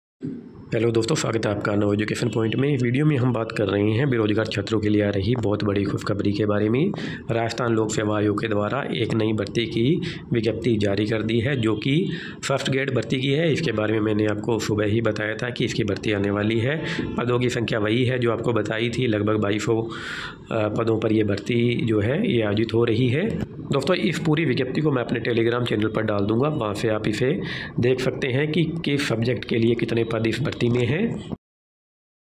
विज्ञप्ति जारी - बड़ी खुशखबरी | राजस्थान लोक सेवा आयोग से शानदार बड़ी खबर नई भर्ती
Is video me rajasthan ki new vacancy ke notification se related update di hai